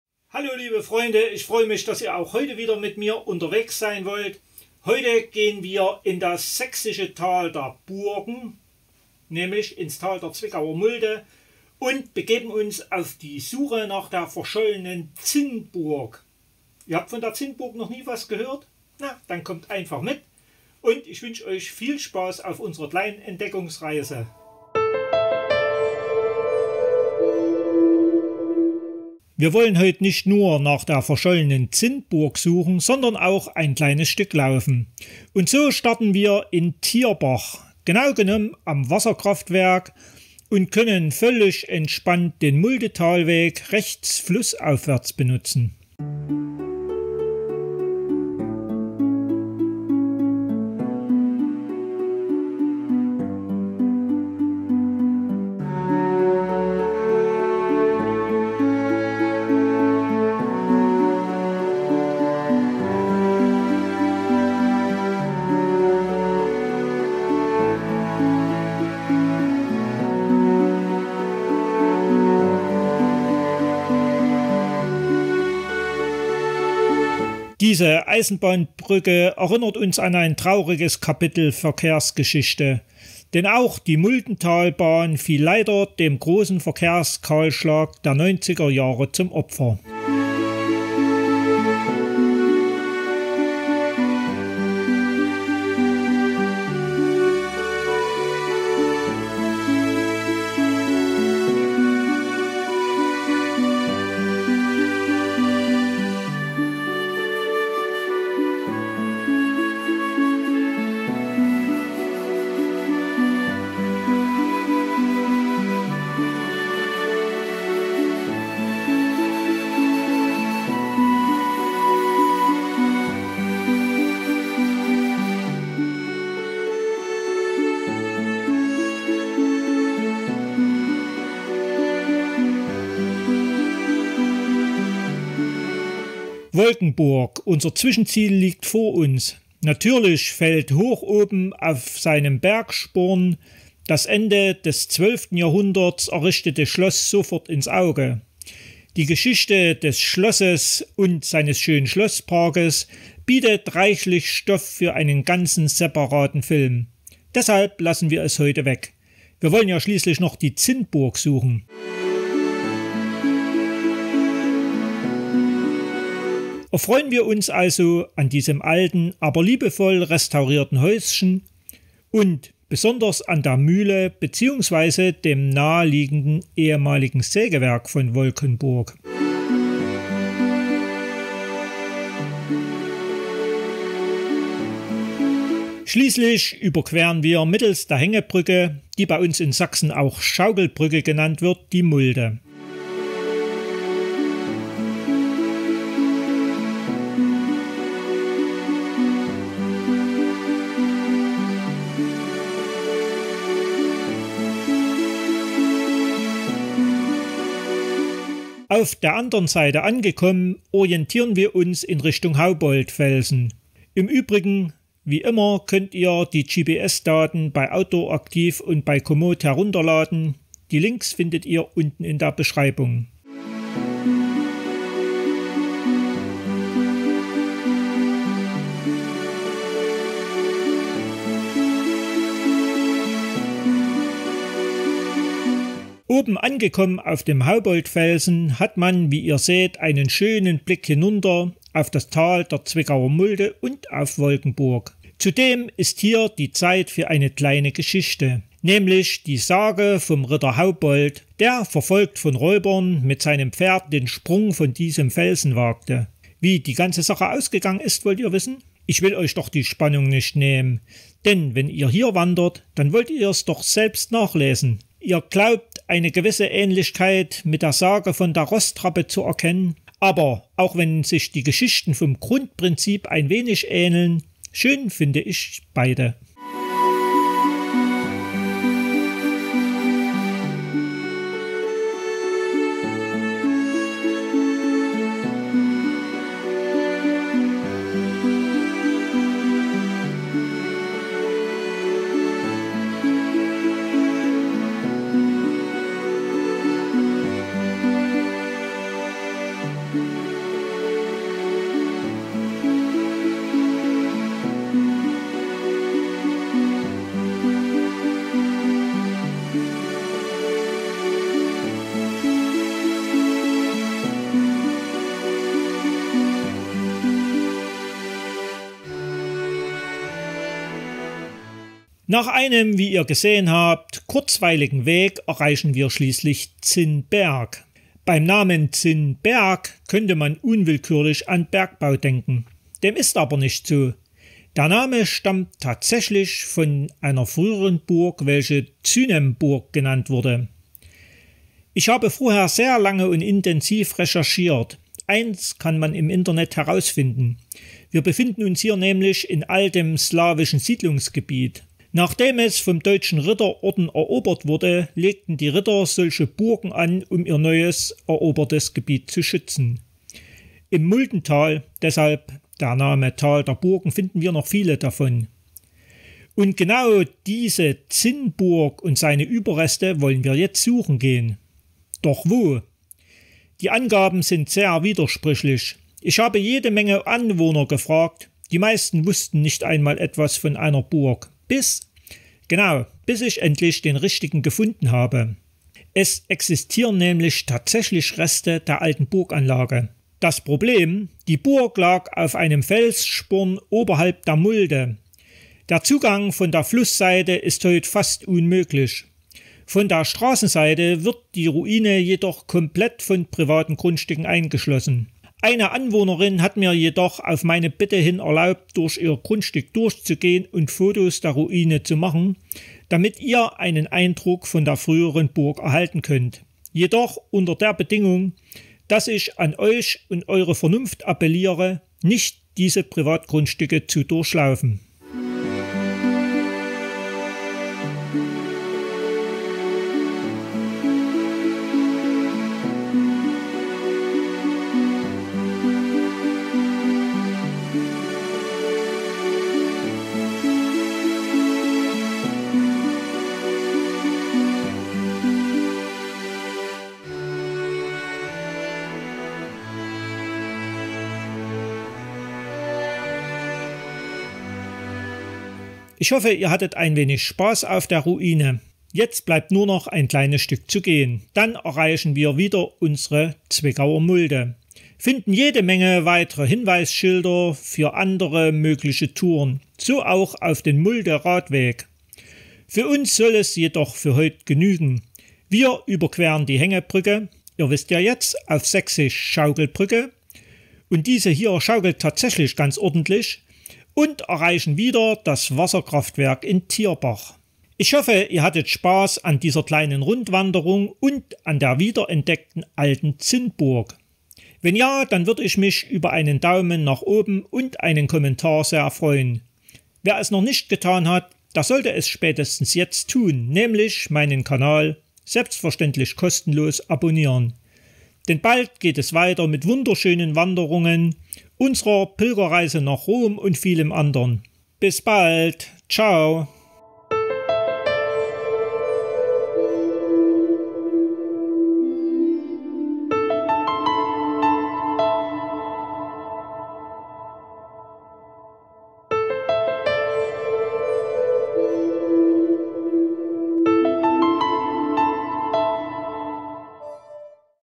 Im Tal der Zwickauer Mulde - auf der Spur der verschollenen Zinnburg
Heute gehen wir in das sächsische Tal der Burgen, nämlich ins Tal der Zwickauer Mulde und begeben uns auf die Suche nach der verschollenen Zinnburg. Ihr habt von der Zinnburg noch nie was gehört? Dann kommt einfach mit und ich wünsche euch viel Spaß auf unserer kleinen Entdeckungsreise.

Today we will go to the Saxon valley of castles, namely the valley of the Zwickauer Mulde, and set out in search of the lost tin castle. You have never heard of the Zinnburg? Then just come along with us and I wish you a lot of fun on our little journey of discovery.
Aujourd'hui, nous allons nous rendre dans la vallée des châteaux de Saxe, à savoir la vallée de la Zwickauer Mulde, à la recherche du Zinnburg perdu. Vous n'avez jamais entendu parler du Zinnburg ? Alors, venez avec nous et je vous souhaite beaucoup de plaisir dans notre petite expédition.
Hoy iremos al valle sajón de los castillos, el valle del Zwickauer Mulde, en busca del Zinnburg perdido. ¿Nunca has oído hablar del Zinnburg? Entonces ven con nosotros y te deseo mucha diversión en nuestra pequeña expedición.
Hoje iremos ao vale dos castelos saxões, ou seja, o vale do Zwickauer Mulde, em busca do Zinnburg perdido. Você nunca ouviu falar do Zinnburg? Então venha conosco e eu lhe desejo muita diversão em nossa pequena expedição.
Oggi ci recheremo nella valle dei castelli sassoni, la valle dello Zwickauer Mulde, alla ricerca della perduta Zinnburg. Non avete mai sentito parlare dello Zinnburg? Allora venite con noi e vi auguro buon divertimento nella nostra piccola spedizione.
Dziś udamy się do saksońskiej doliny zamków, czyli doliny Zwickauer Mulde, w poszukiwaniu zaginionego Zinnburga. Nigdy nie słyszałeś o Zinnburgu? Więc chodźcie z nami i życzę wam dobrej zabawy na naszej małej wyprawie.
Сегодня мы отправимся в саксонскую долину замков, а именно в долину Цвикауэрской мульды, в поисках затерянного Циннбурга. Ты никогда не слышал о Зиннбурге? Тогда просто пойдем с нами и я желаю тебе много веселья в нашей маленькой экспедиции.
Dnes jdeme do saského údolí hradů, konkrétně do údolí Zwickauer Mulde, a vydáme se hledat ztracený Zinnburg. Nikdy jste neslyšeli o Zinnburgu? Pak stačí přijít a doufám, že si naši malou cestu za poznáním užijete.
今日は、失われたジンブルクを求めて、城のサクソンの谷、すなわちツヴィッカウアー・ムルデの谷に行きます。ジンバーグって聞いたことないの？私たちと一緒に来て、私たちの小さな遠征が楽しくなることを祈っています。
今天我们将前往萨克森城堡谷，即茨威考尔穆德谷，寻找失落的津恩堡。你没听说过津堡吗？那就跟我们一起去吧，祝你在我们的小探险中玩得开心。

- Musik Intro: Thomas Fenner

0:00 Intro
0:30 Filmstart
2:16 Wolkenburg
3:56 Hauboldfelsen
4:36 weiter gehts
5:27 Zinnberg
6:26 Zinnburg
7:40 wir laufen weiter